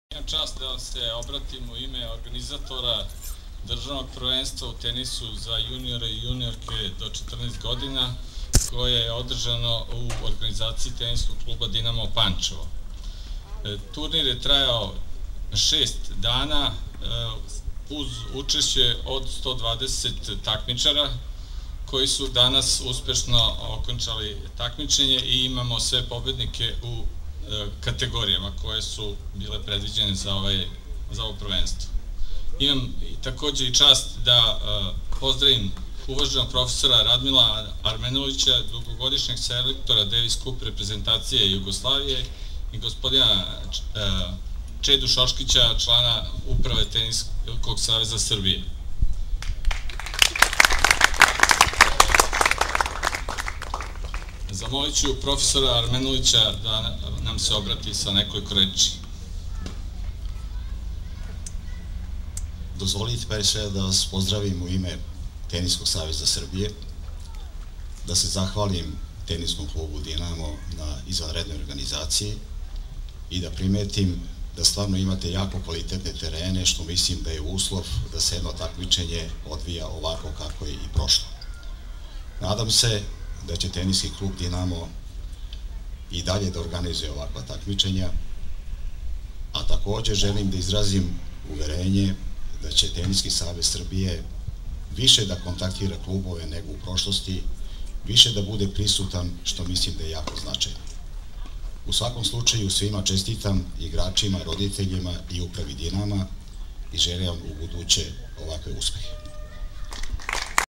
Летње првенство Србије у Панчеву 2014, ТК Динамо
Летње првенство Србије у Панчеву за дечаке и девојчице до 14 година, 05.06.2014; додела награда